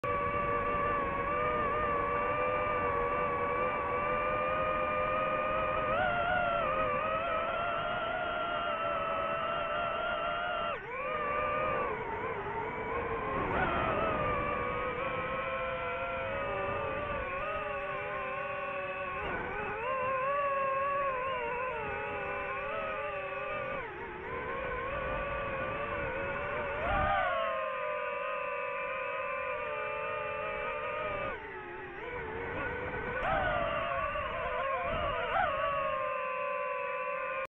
DJI FPV Goggles records the motors...
The HD from the drone itself records excellent video while the goggles record lower quality files like that of a smart phone but with audio.  I was really impressed when I played it because I love the way it sounds.